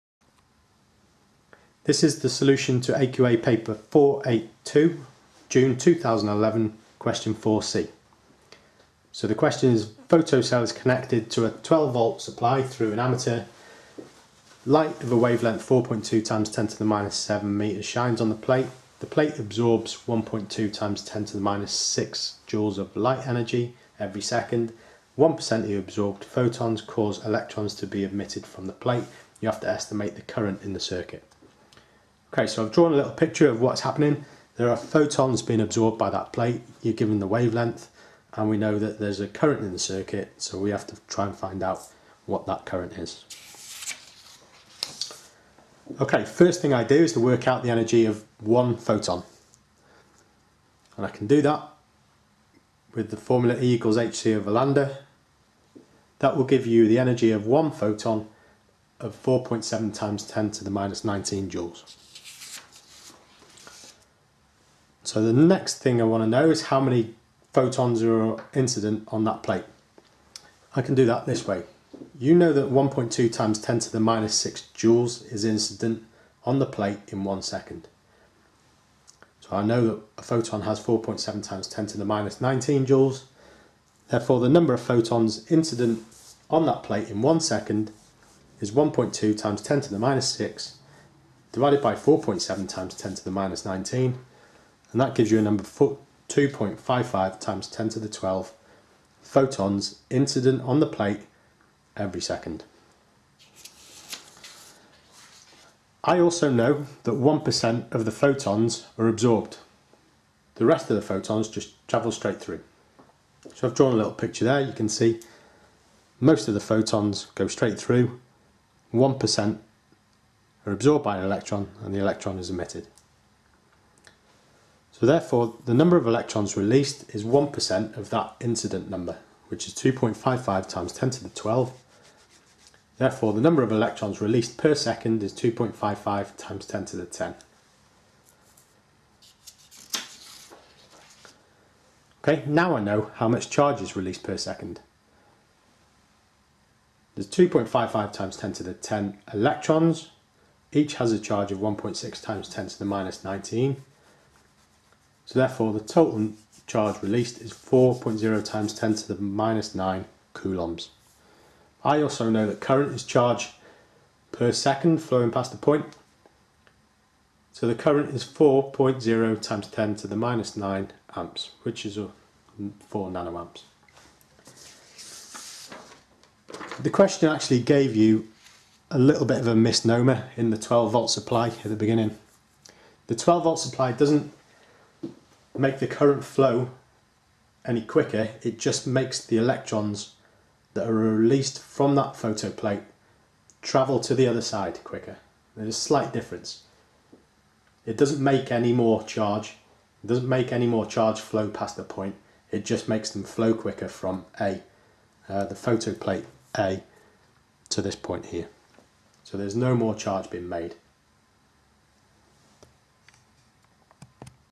G482 Jun 2011 Q4(c).MP4
Solution to June 2011 - Electrons Photons Waves - 4(c)